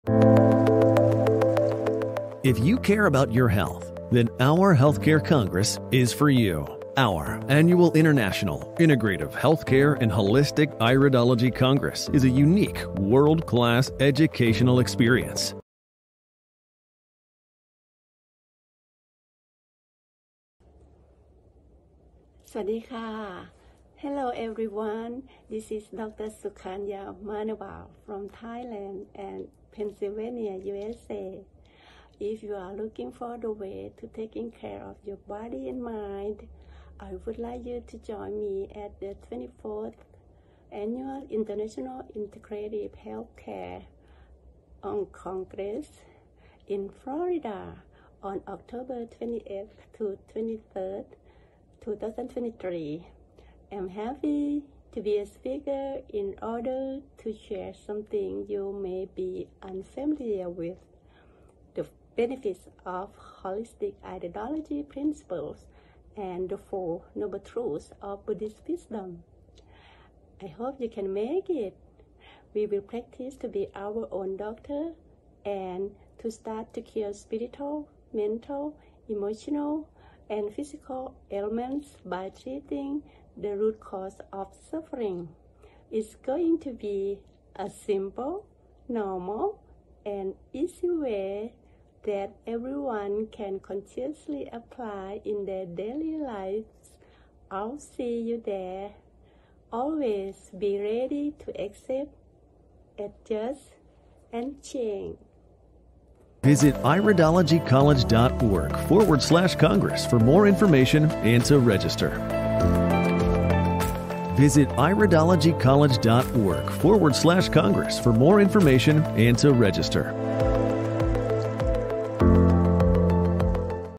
Meet Dr. Sukanya Manobal, Speaker at "OUR" 24th Annual Int'l Integrative Healthcare & Holistic Irido
Meet Dr. Sukanya Manobal ~ Thailand, Speaker at "OUR" 24th Annual Int'l Integrative Healthcare and Holistic Iridology Congress, October 20-22 (Fri-Sun) plus Workshops on Monday Oct 23rd in Orlando, Florida, USA.

October 20-23, 2023 Congress (Fri-Sun) + Full day Workshops on Monday

Venue: Orlando Airport Marriott Lakeside Hotel
City: Orlando, Florida, USA